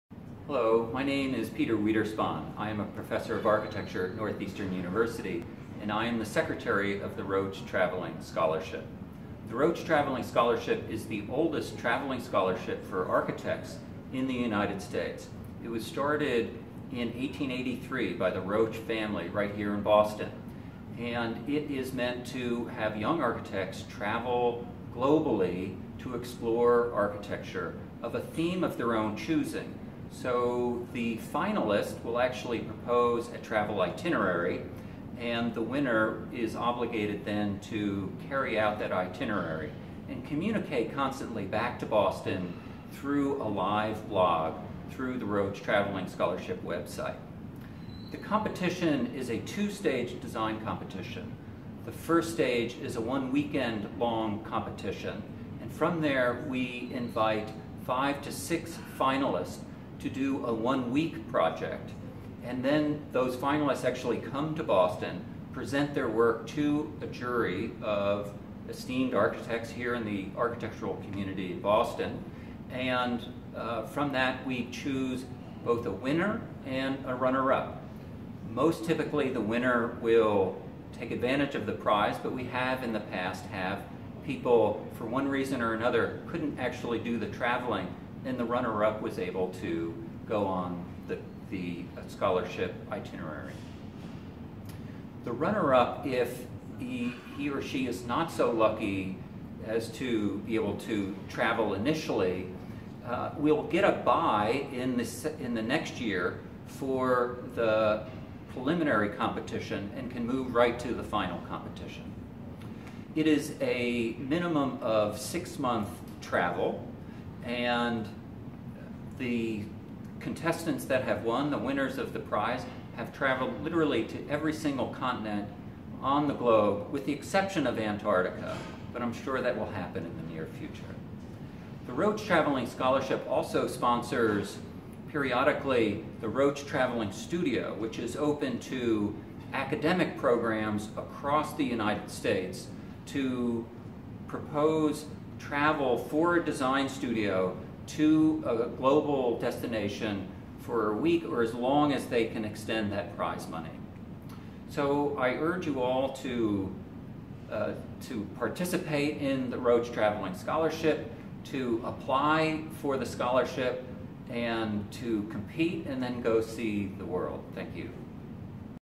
About the Rotch Travelling Scholarship with Peter Wiederspahn AIA
The Rotch Travelling Scholarship grants one winner annually the funds to travel internationally and investigate architecture for at least six months.